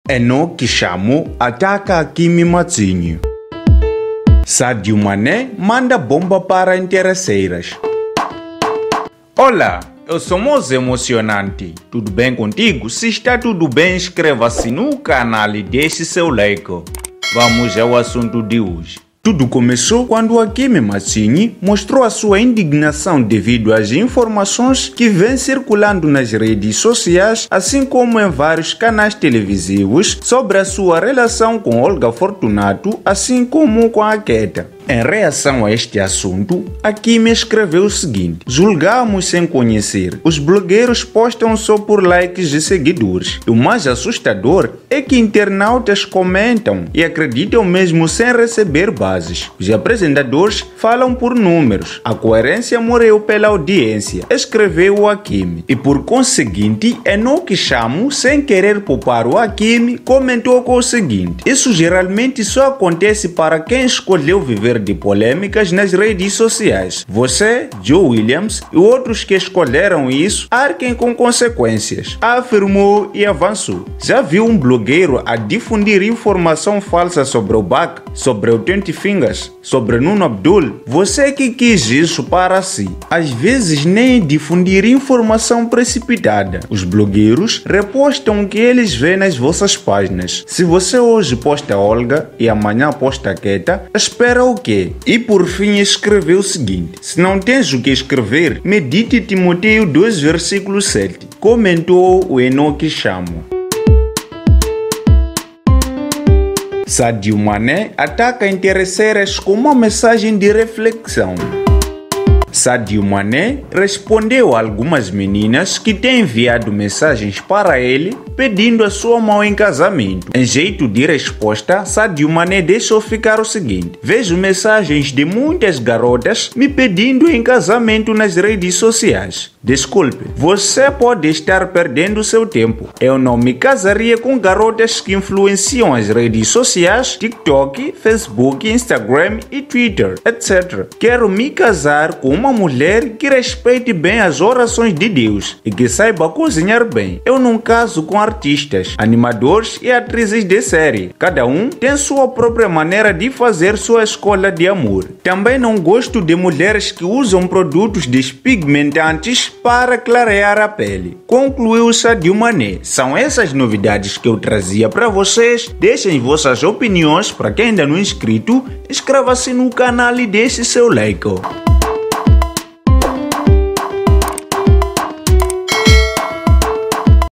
BOMBA: SÁDIO MANÉ ATACA AS MULHERES & ENOQUE CHAMO ATACA AKEEM
SÁDIO MANÉ ATACA AS MULHERES & ENOQUE CHAMO ATACA AKEEM

Eneque Chamo diz que o que acontece com Akeem ele é que procurou por isso tem que evitar reclamar.

FONTE: Moz Anuncia Só Novidades
         Africa & Celebridades